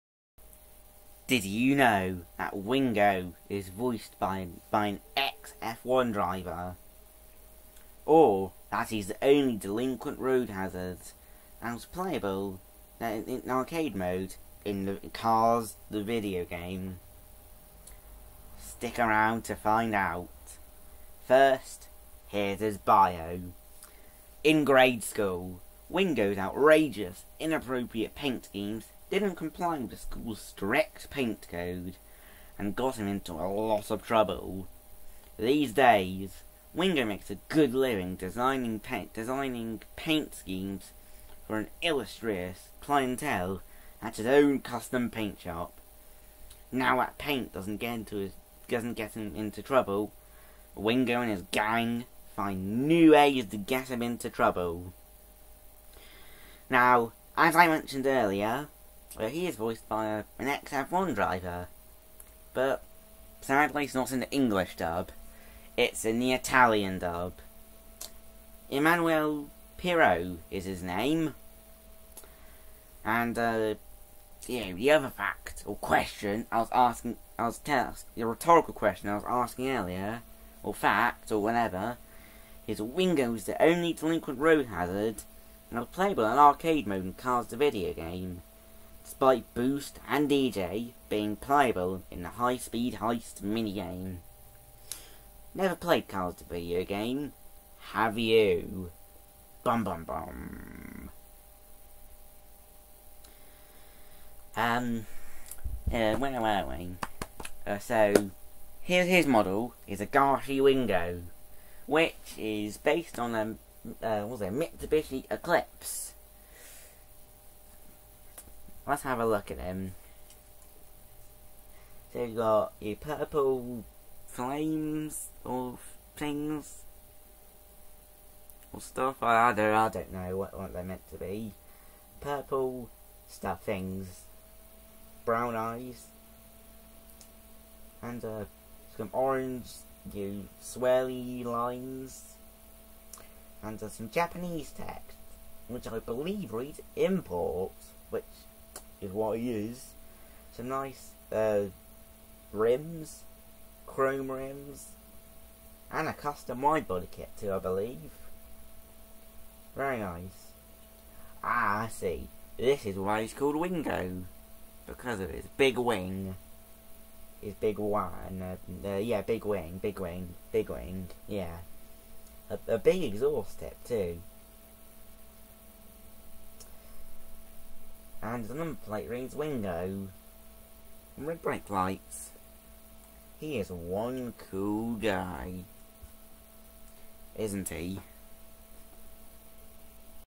Wingo Review (Cars 1)
Here's my review of Wingo from Cars 1. He is based on a Mitsubishi Eclipse.

Review inspired by: @MrDocket